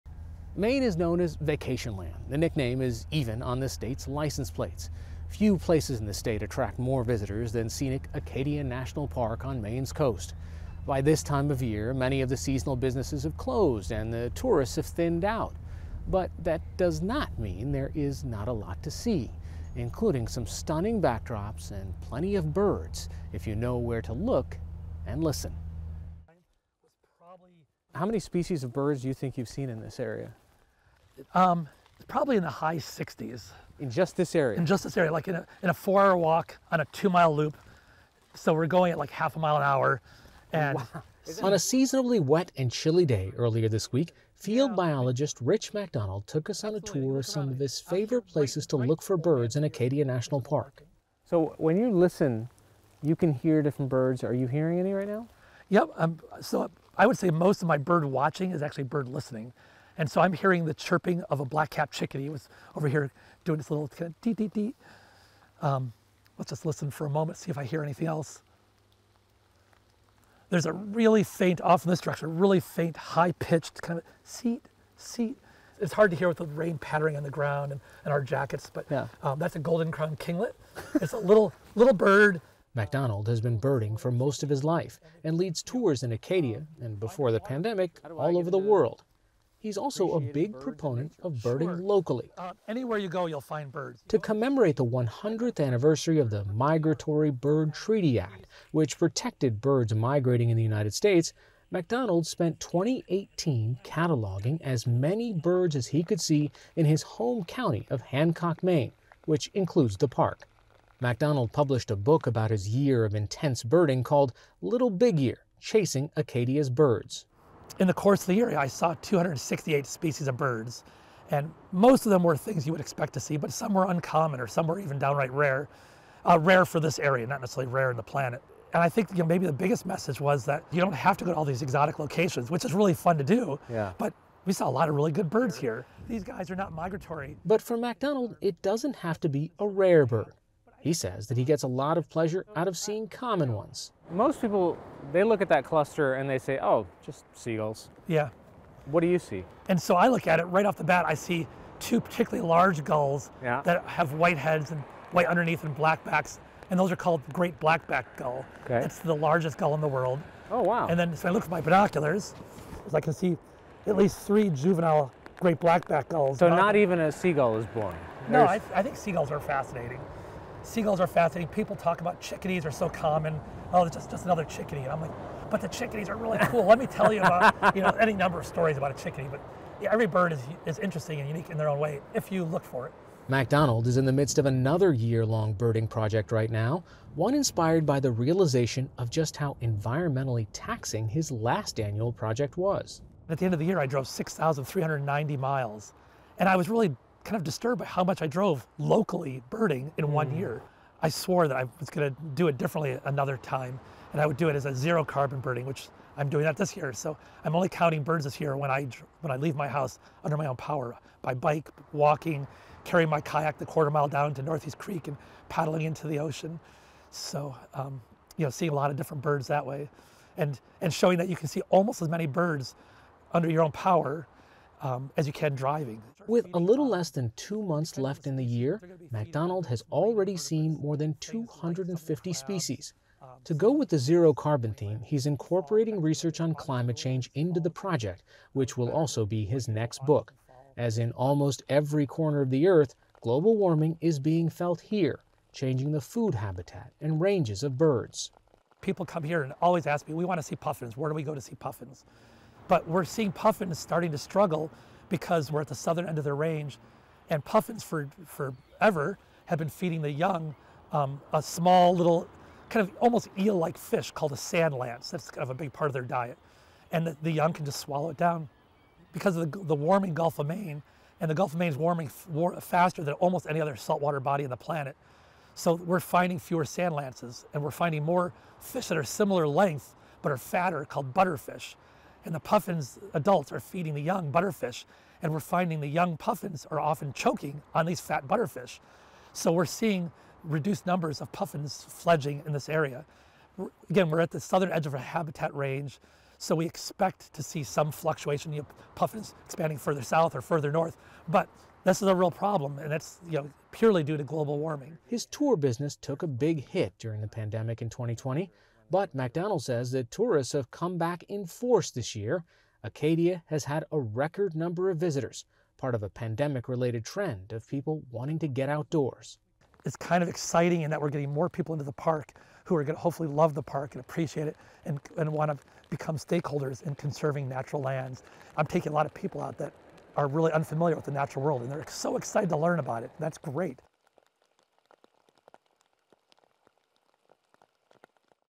A year in the life of Acadia National Park's birds
For a year, Rich MacDonald cataloged every bird he saw in his home county of Hancock, Maine, which includes the famous Acadia National Park. His so-called ‘big year’ is cataloged in his book, “Little Big Year, Chasing Acadia’s Birds.” MacDonald is now embarking on a new project to see as many birds as he can, by foot or by bike, while describing how climate change is already dramatically changing the ecology of this wild, iconic place. Hari Sreenivasan reports.